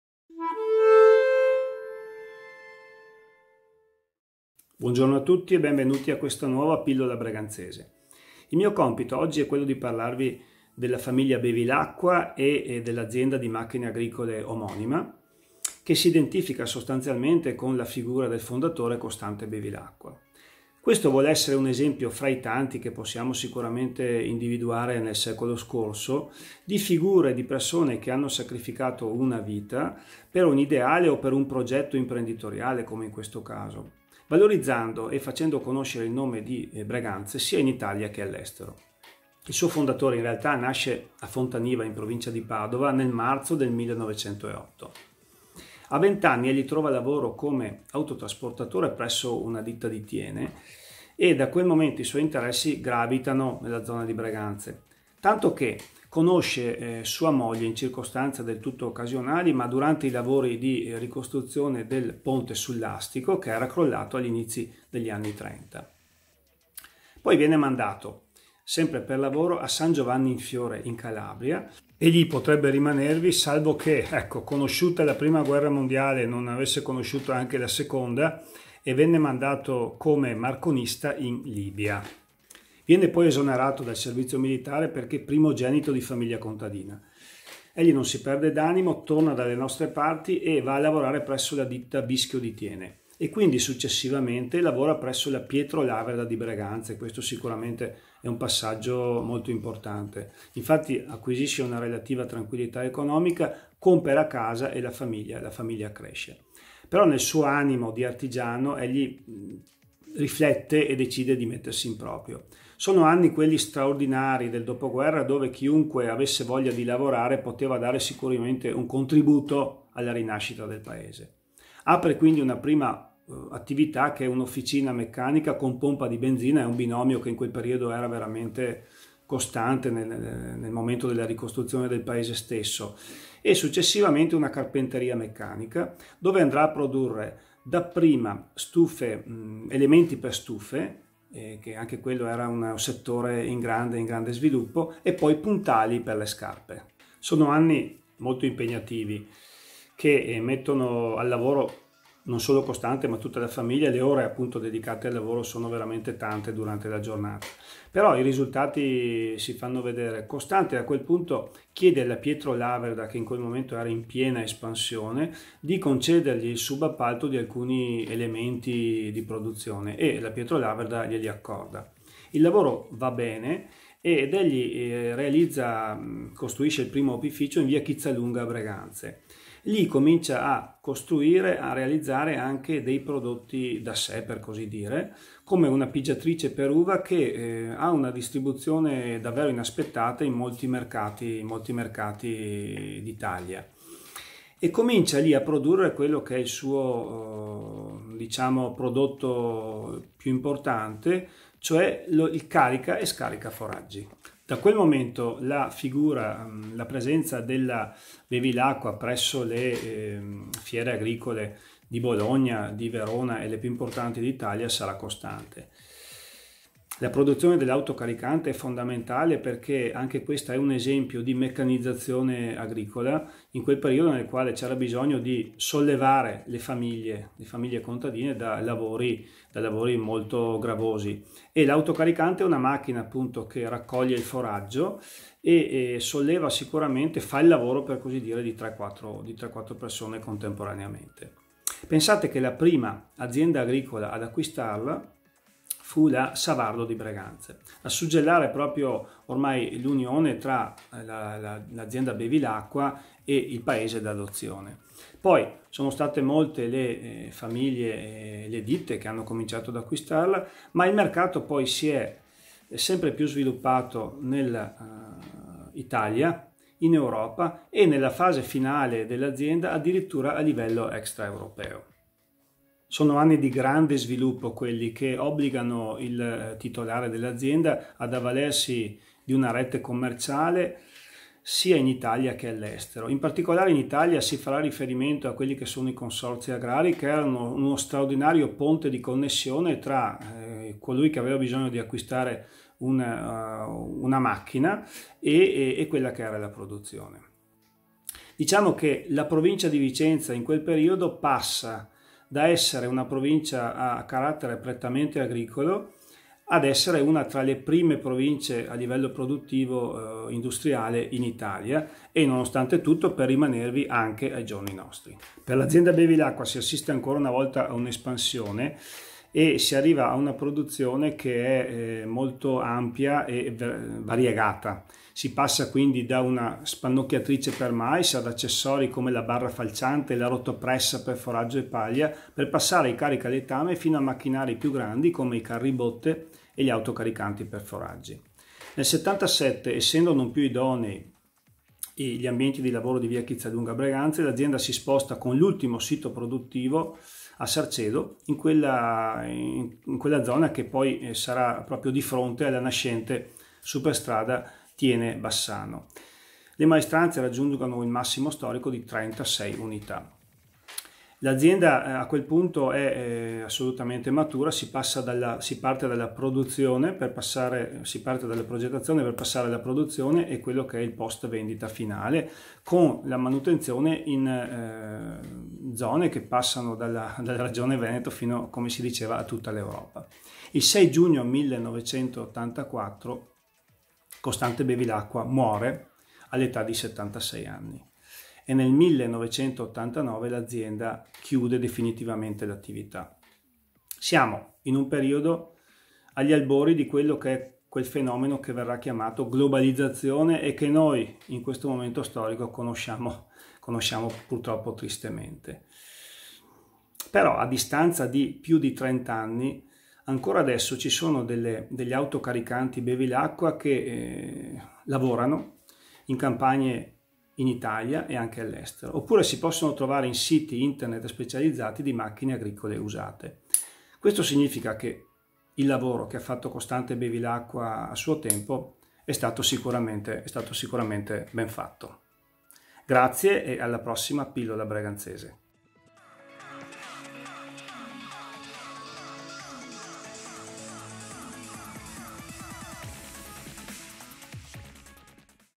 Pillole di storia breganzese - Le macchine agricole Bevilacqua
Renato Guglielmi ci riporta la parabola della Bevilacqua, azienda a lungo impegnata nella produzione di macchine agricole